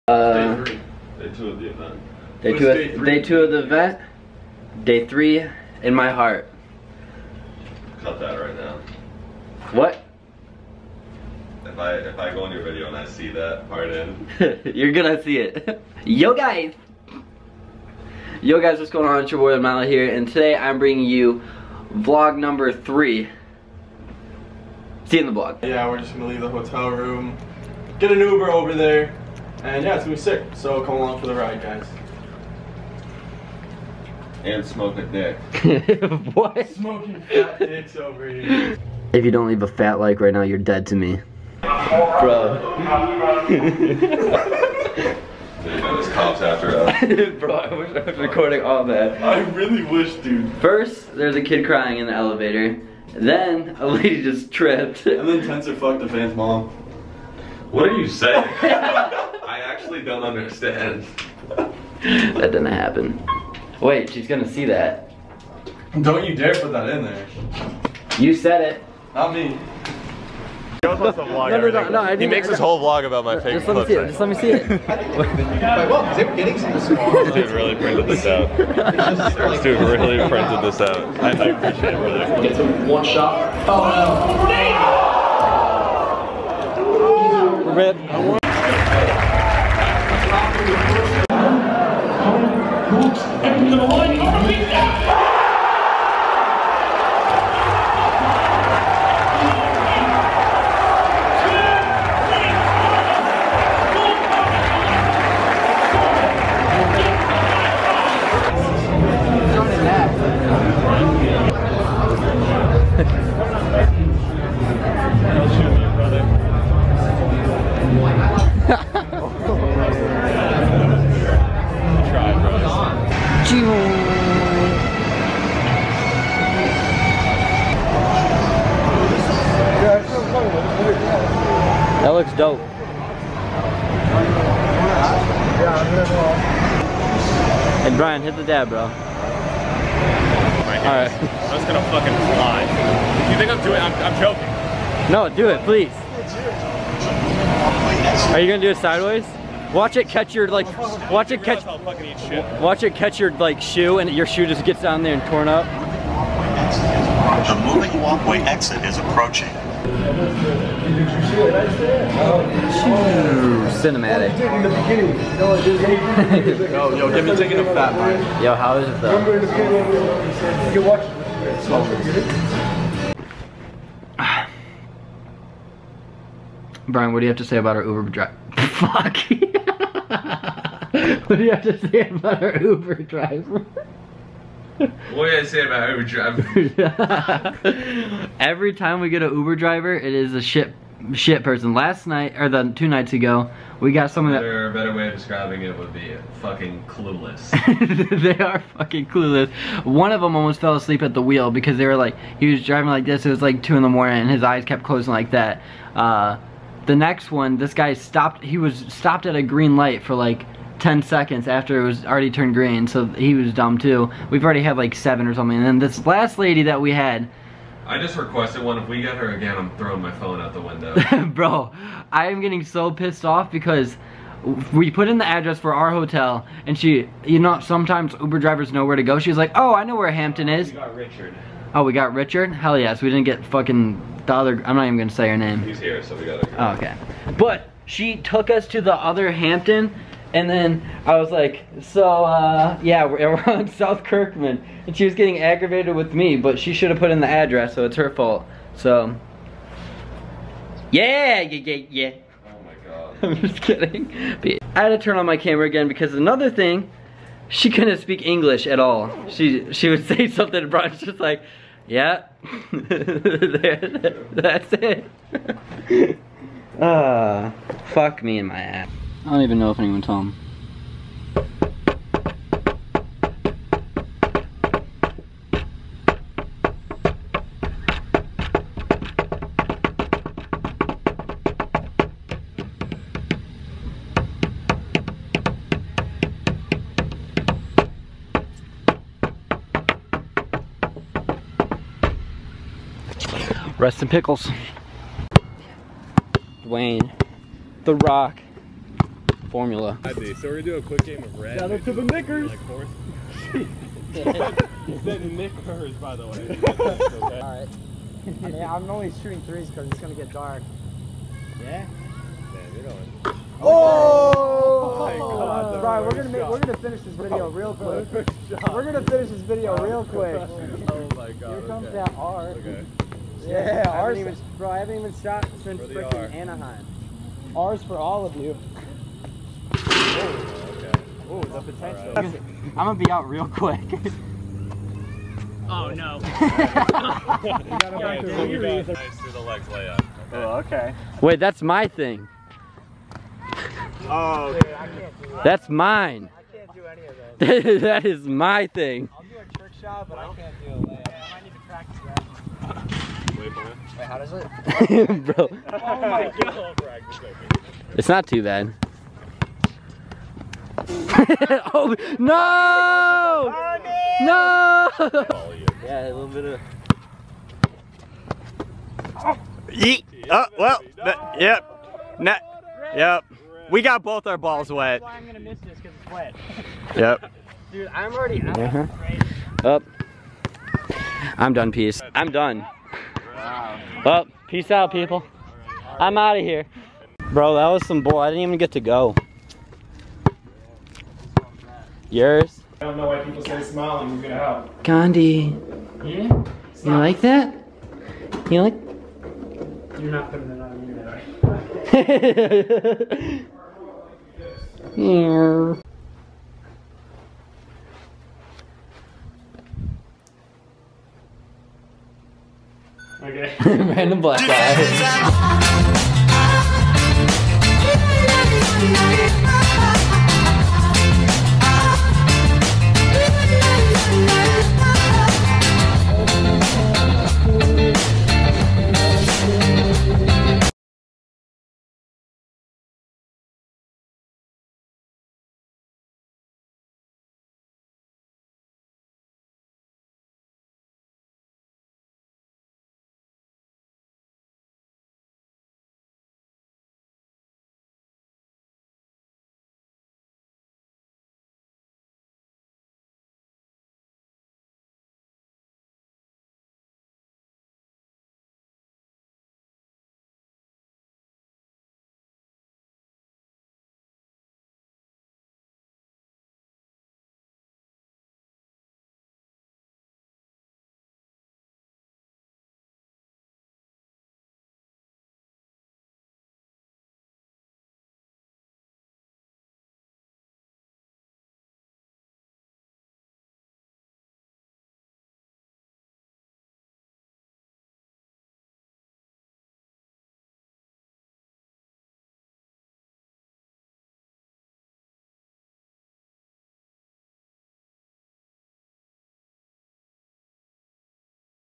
TENSER'S MISSED FRAME!? (Worst Uber Drivers Ever)
A fan really brought Tenser his missed frame lol. Plus fuck those Uber's